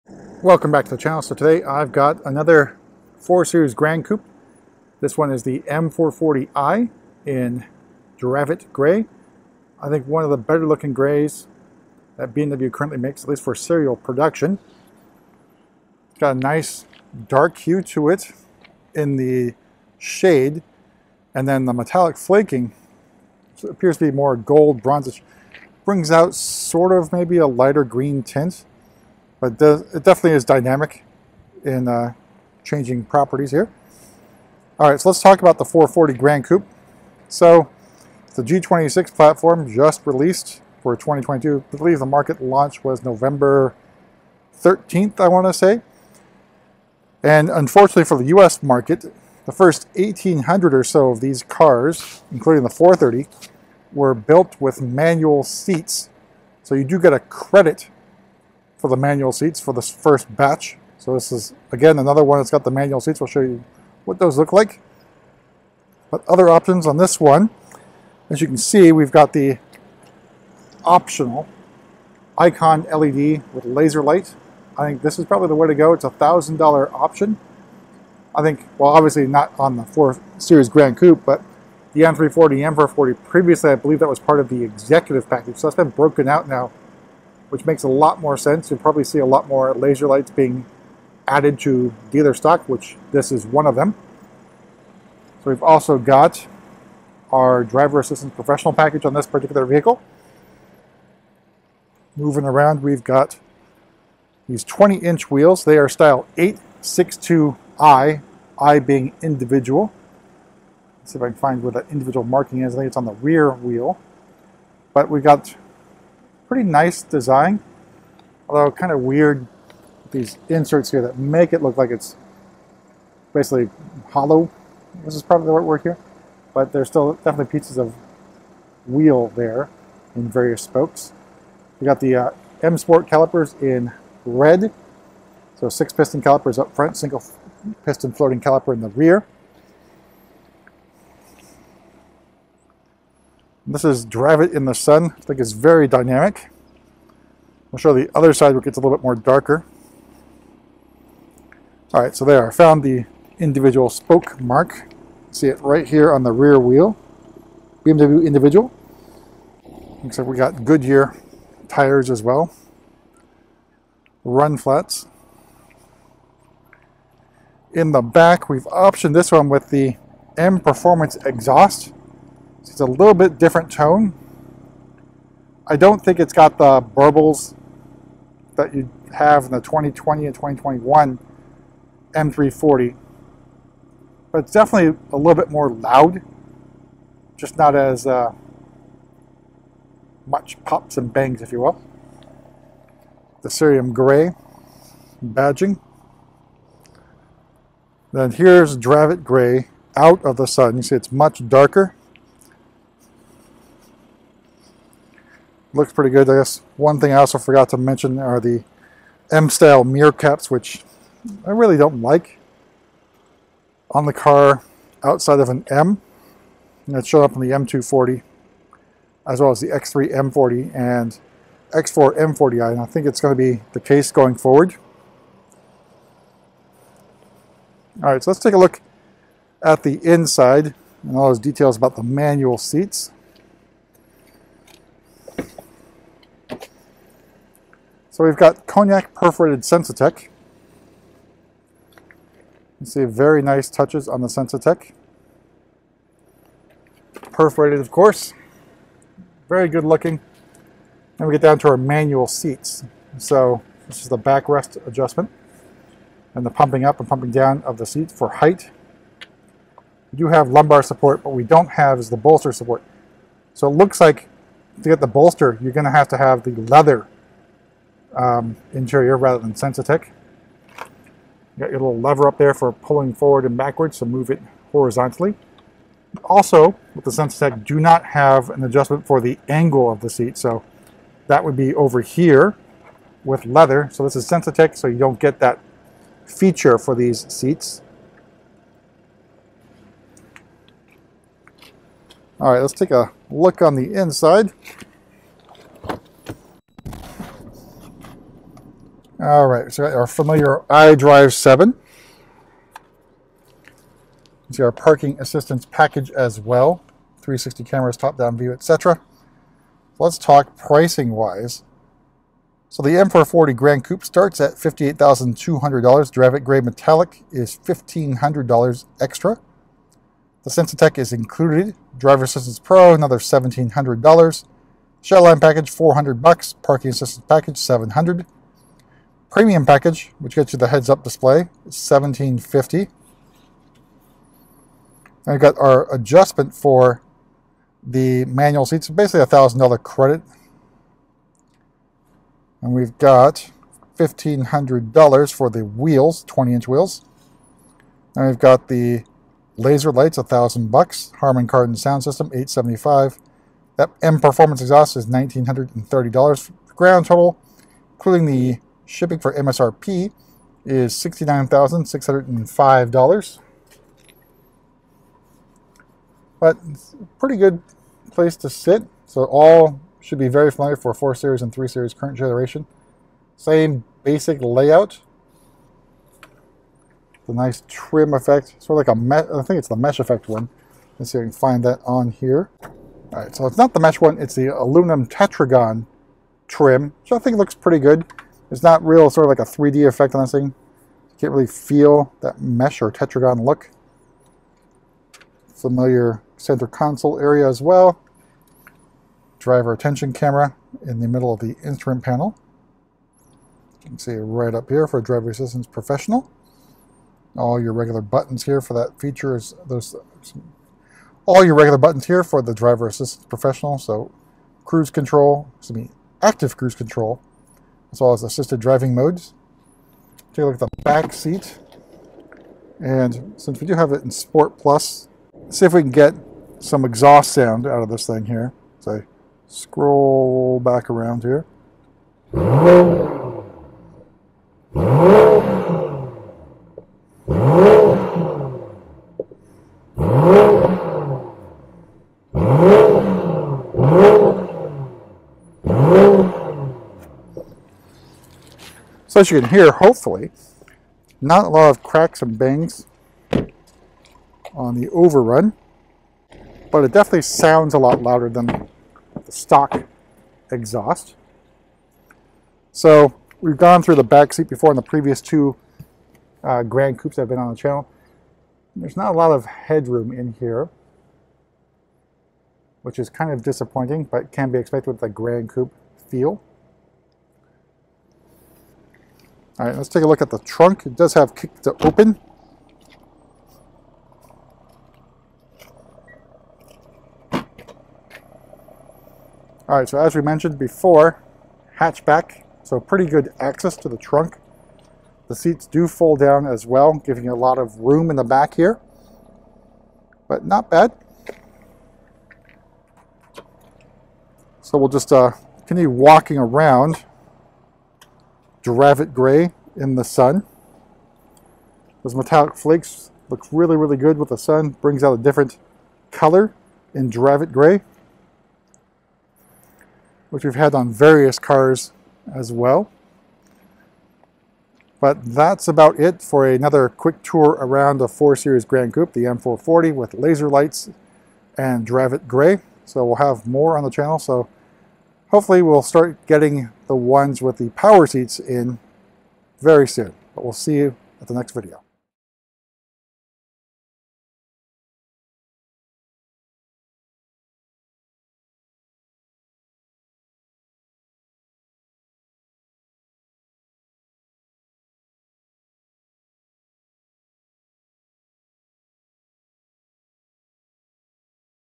Tour the M440i Gran Coupe in Dravit Grey | 4K
Yet another M440i Gran Coupe has arrived sans power seats, but we added the M Performance Exhaust to this one. Louder for sure, but no pops and bangs.